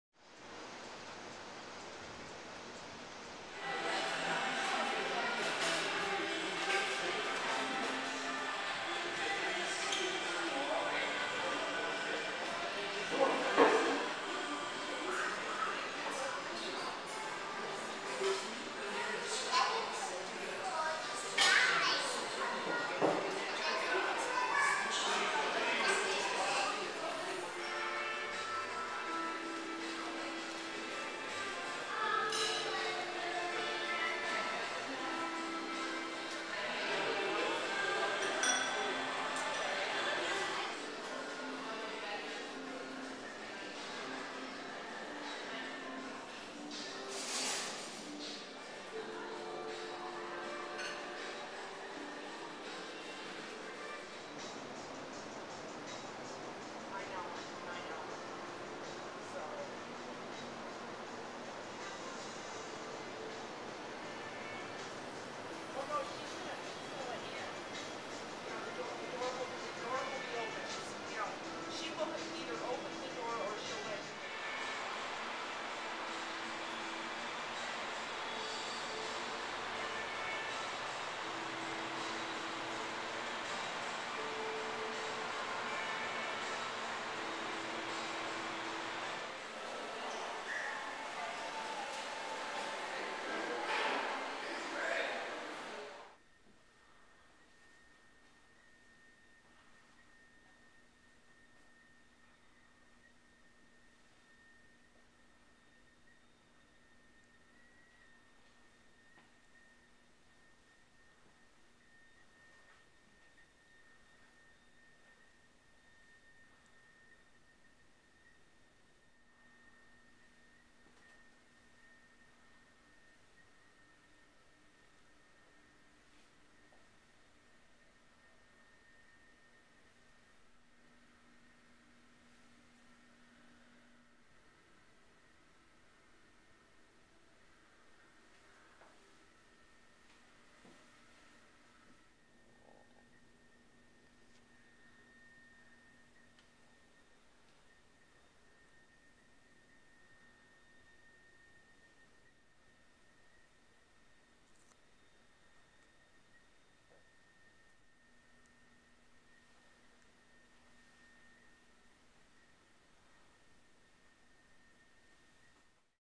Element by Westin - Extended Stay Hotel in Lexington, MA
A look at the newly opened Element hotel in Lexington, Massachussets